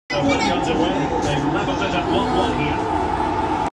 Gaming with an audience is always better !!
And the stage is getting hotter as more gamers step up to challenge Dodo and win great prizes. Hurry up and come to the Mashreq Neo NXT Gaming Truck at the Ripe Market, Academy Park if you wish to match skills with her today.